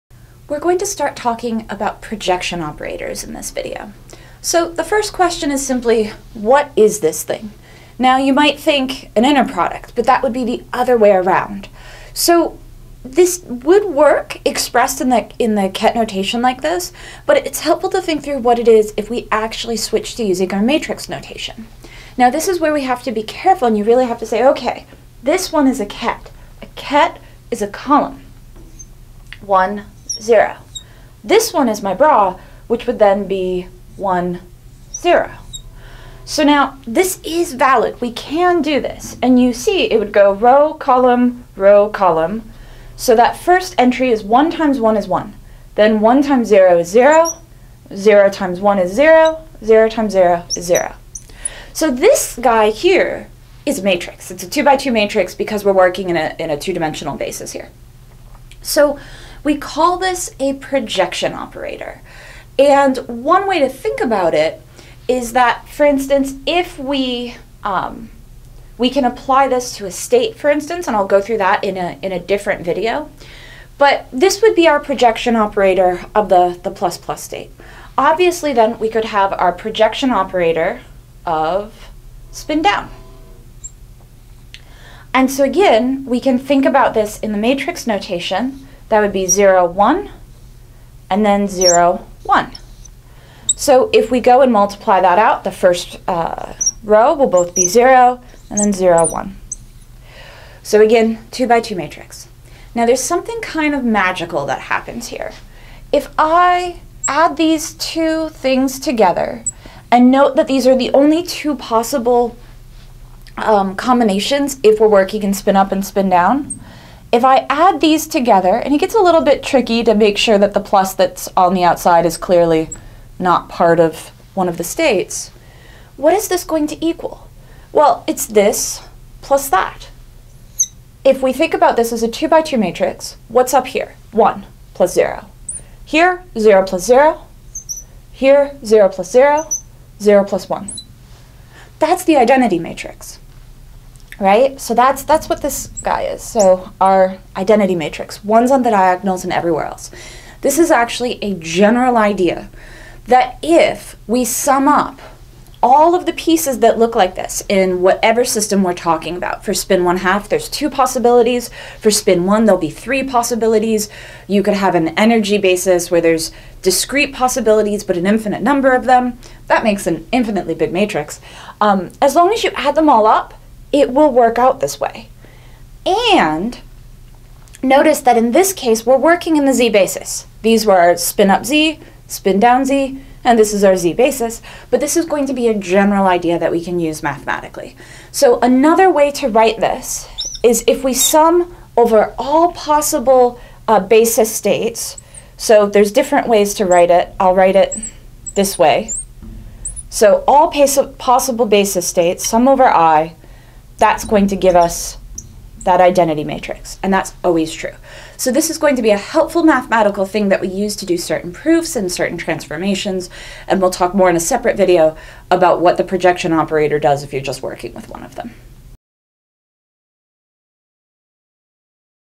Projection Operators in matrix notation
Creating the matrix representation of projector operators from the ket-bra definitions.